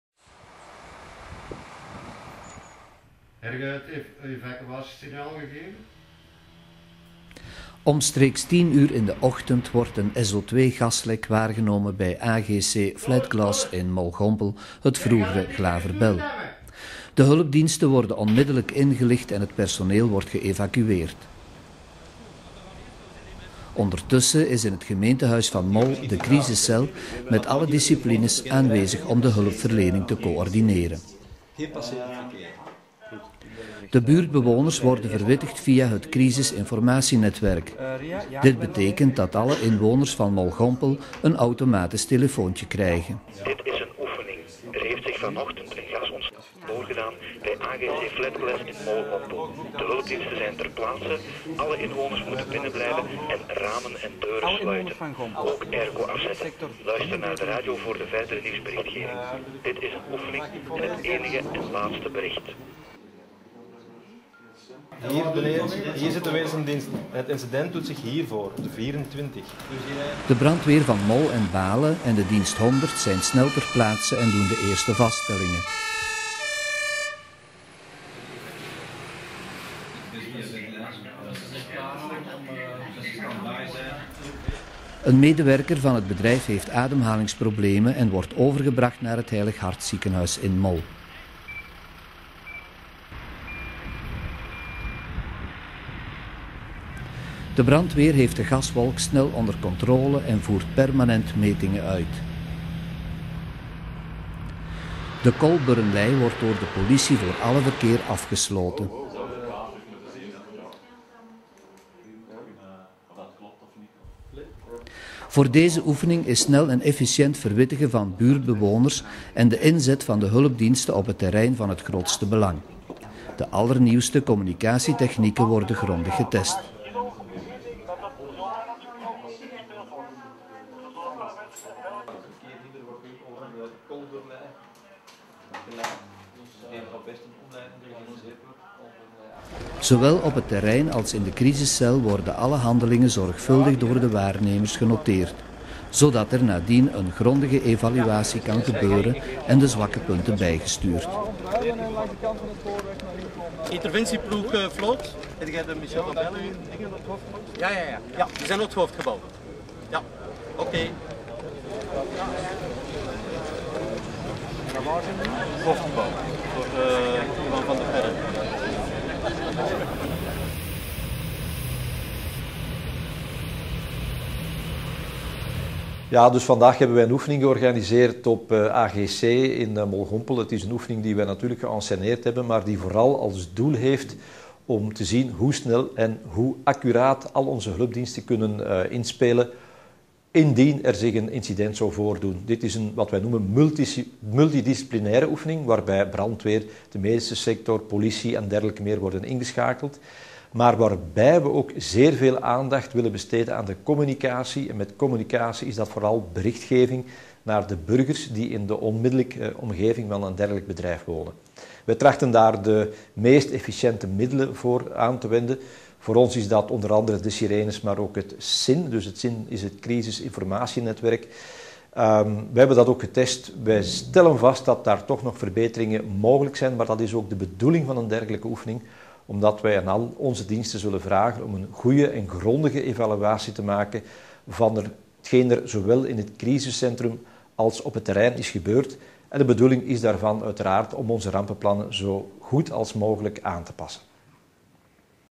Rampoefening in MOL
AFM.
Om de samenwerking van de verschillende diensten op elkaar blijvend af te stemmen en bij te sturen, moeten ten gepaste tijden "rampen" gesimuleerd worden. Een grote oefening is deze in samenwerking met get glasfabriek van Mol-Gompel. Het lijkt echt, daarom wordt er ook heel veel van geleerd, om erg te voorkomen, moest het toch eens echt gebeuren.